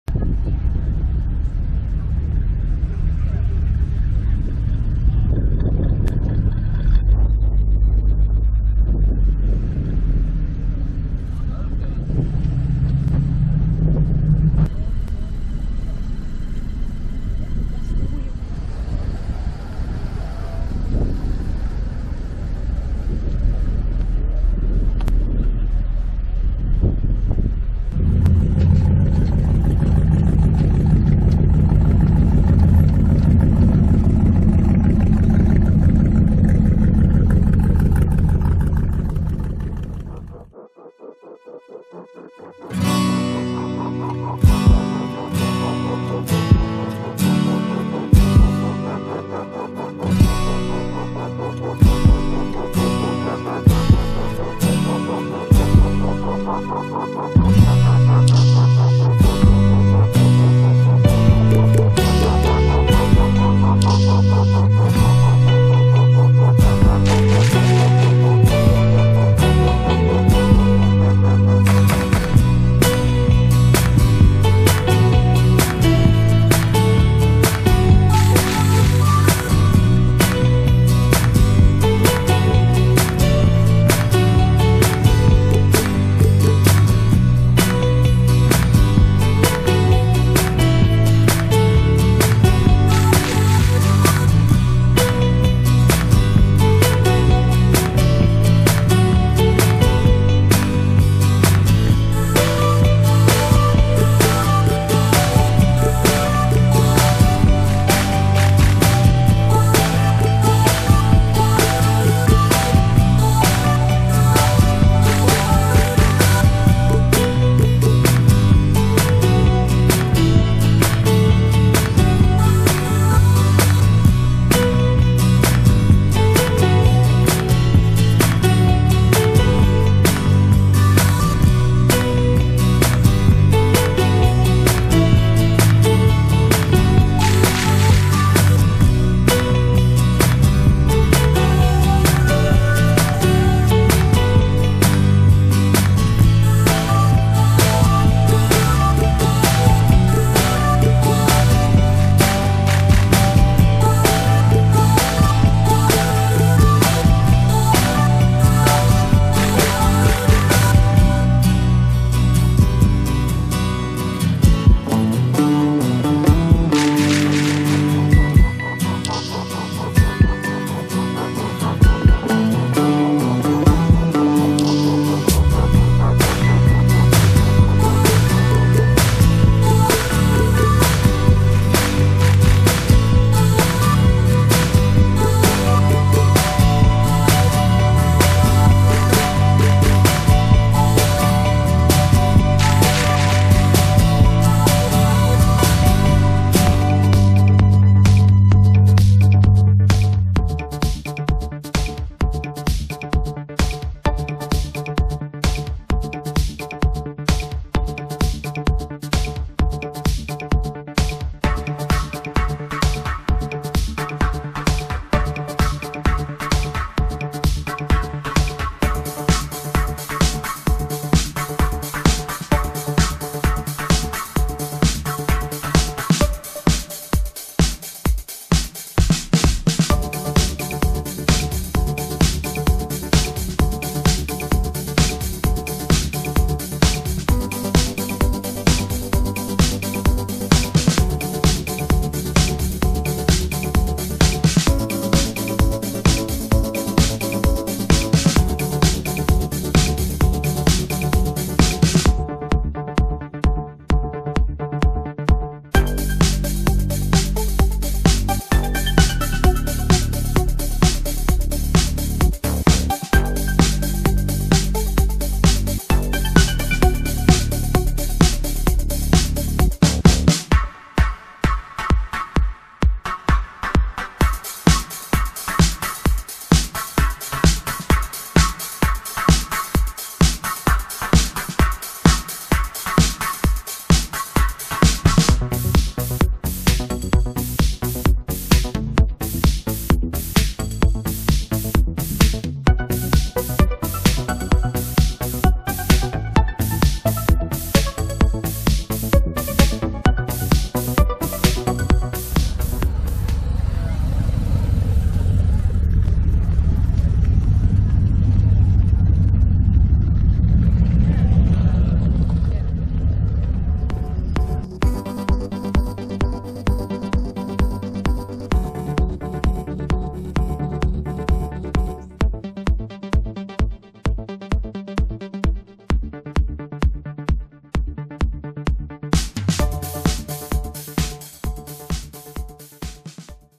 Coffee & Chrome - Chateau Impney - Sunday 9th June 2024
Excellent Event by Footman James Classic Car Insurance.
Amazing selection of vehicles in an iconic location 😀👍